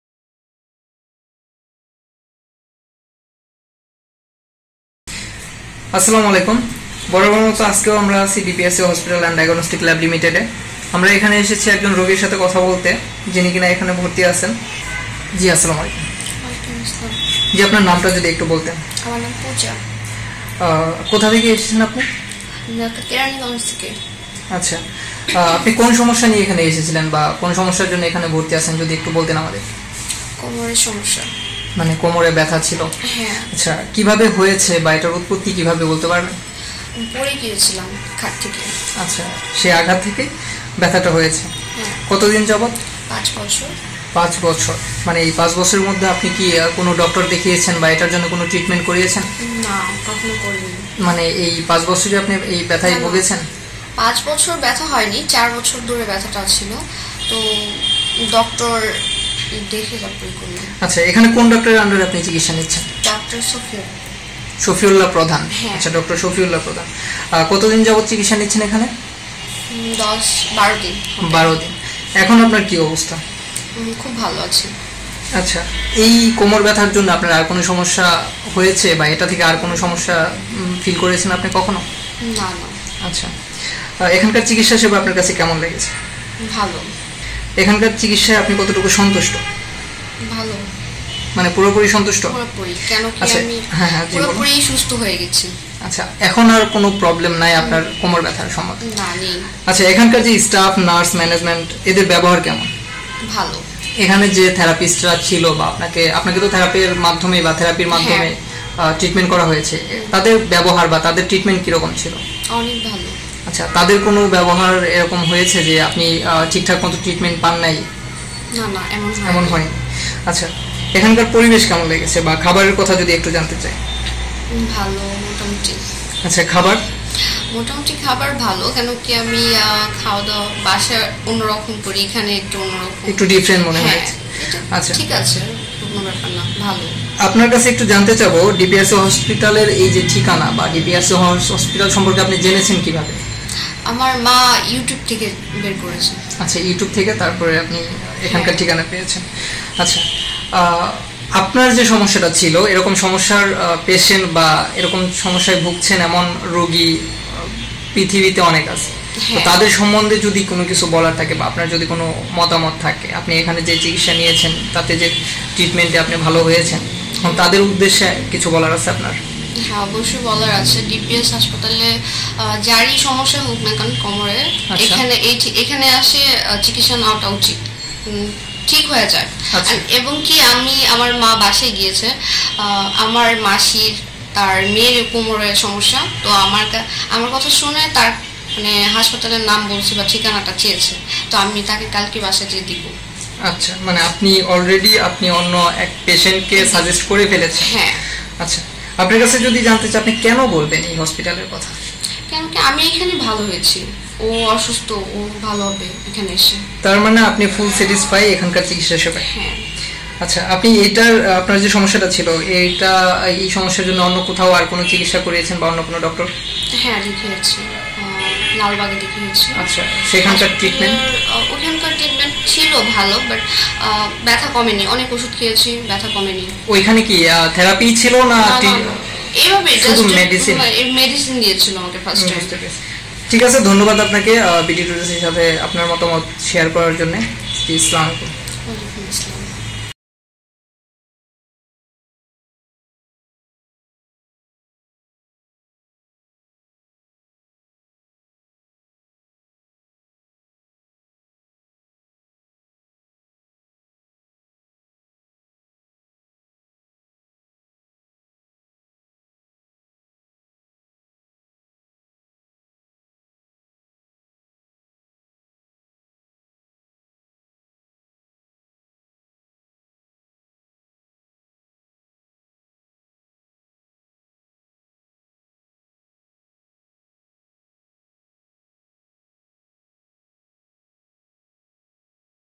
৫ বছর ধরে কোমর ব্যথা রোগী চিকিৎসা নিয়ে সুস্থ হয়ে বাড়ী ফিরলেন।
৫ বছর ধরে কোমর ব্যথা রোগী চিকিৎসা নিয়ে সুস্থ হয়ে বাড়ী ফিরলেন।